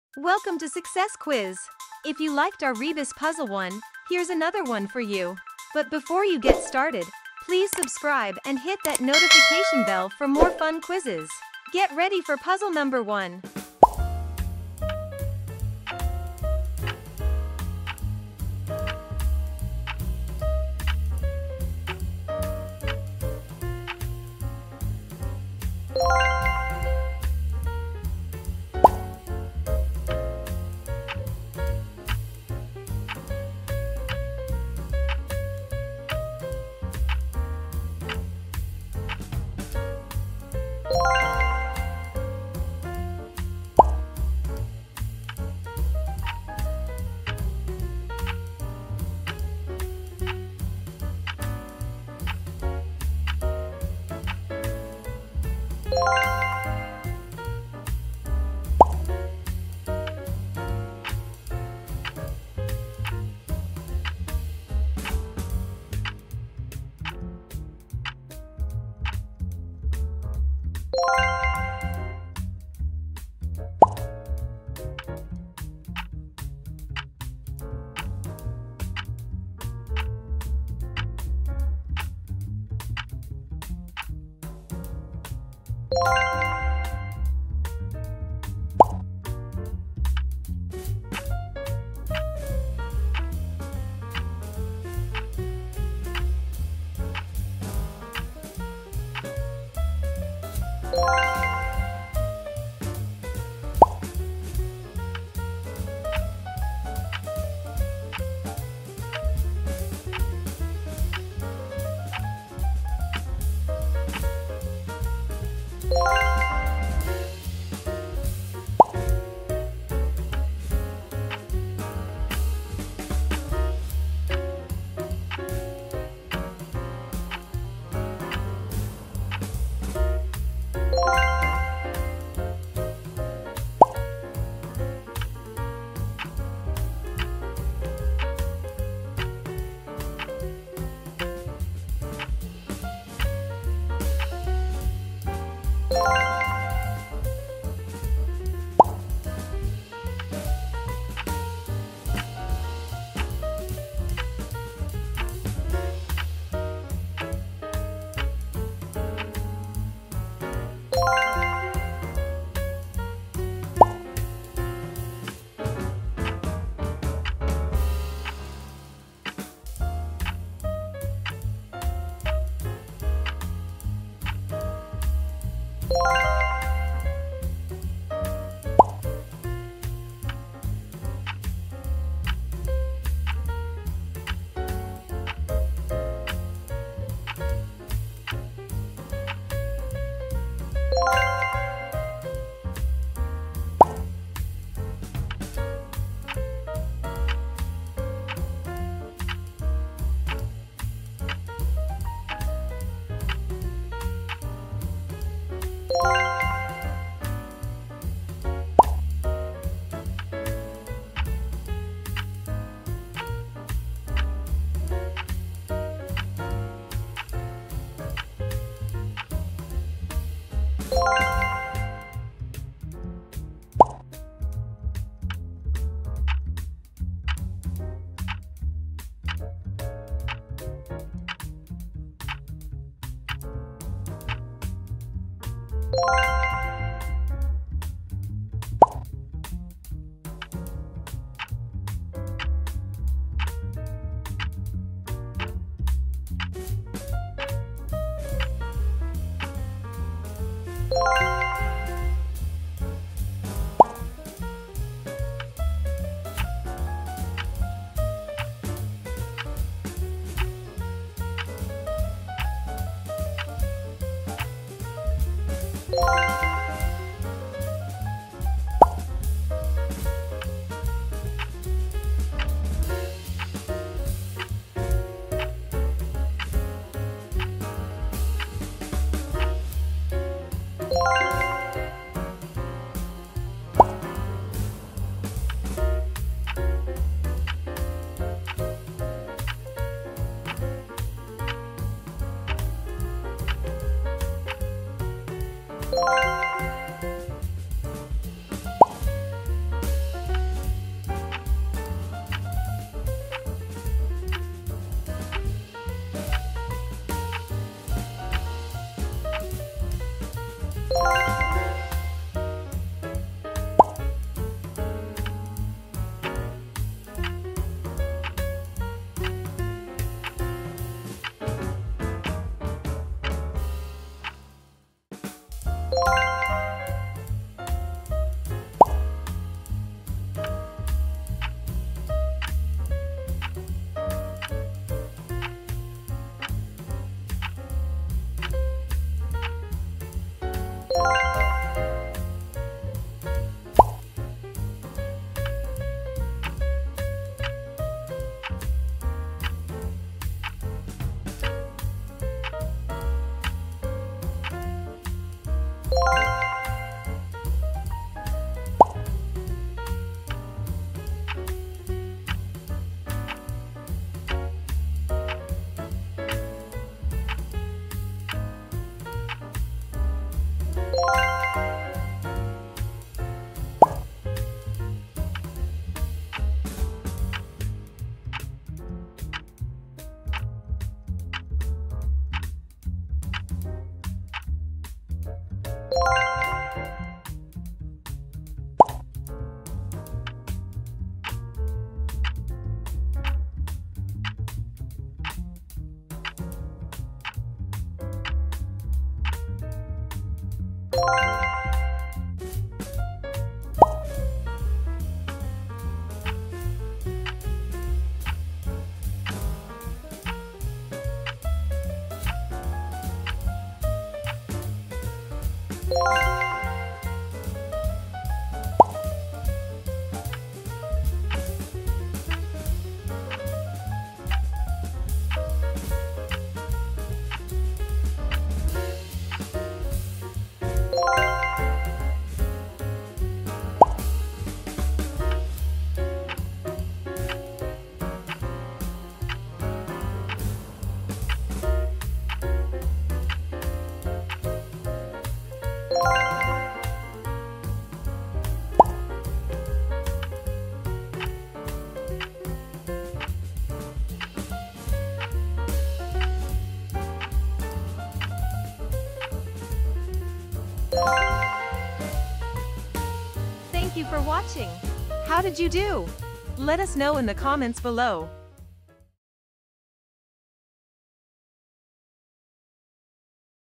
Can You Guess These Word and Picture Riddles? | Rebus Puzzles #2 | Dingbats
Here is another fun word and picture riddle quiz! Can you guess the common word phrases?

How To Play:
- You will see 30 different puzzles.
- Guess the common expressions or idiom.
- You have 10 seconds until the timer runs out.
- After the timer runs out, the answer will be shown.
Have fun! 😊

How many common expressions and idioms do you know? Please share your score and comments below!